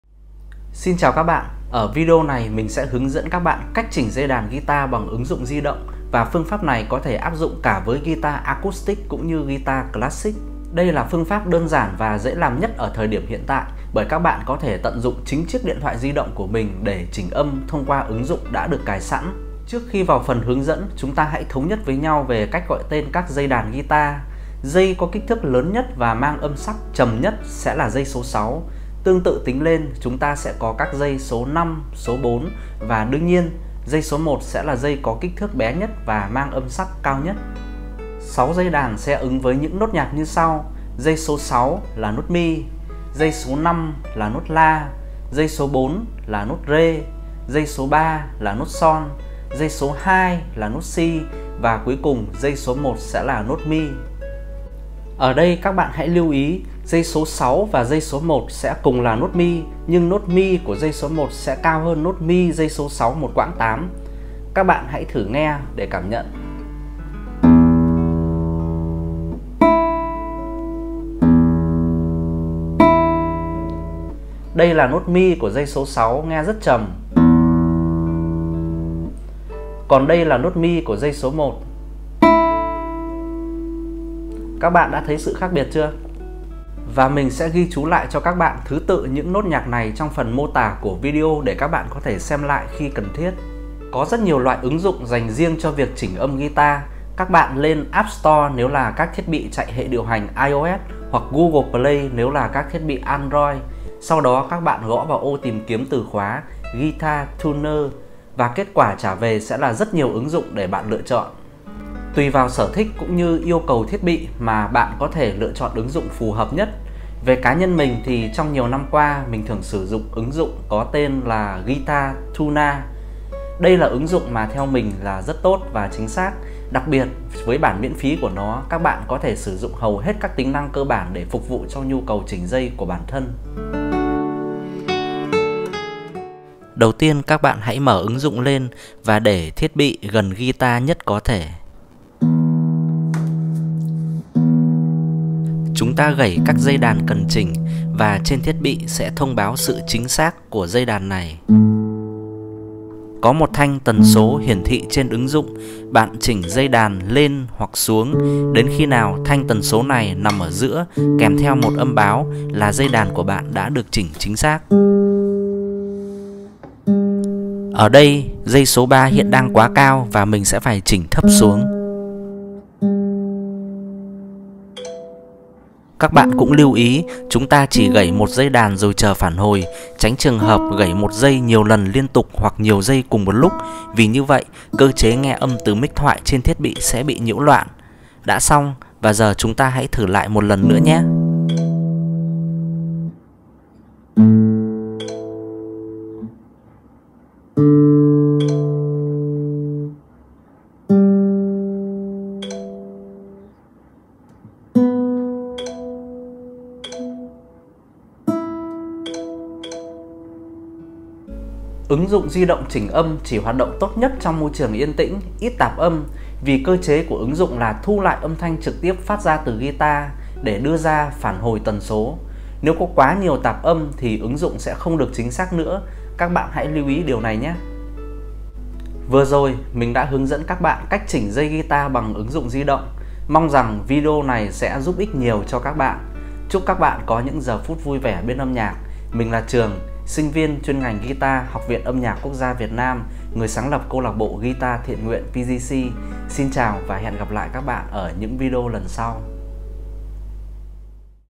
#9 Cách Chỉnh Dây Đàn Guitar Trên Điện Thoại Di Động - App Guitar Tuna | KIẾN THỨC GUITAR
© Copyright by Tran Van Truong (Do not Reup)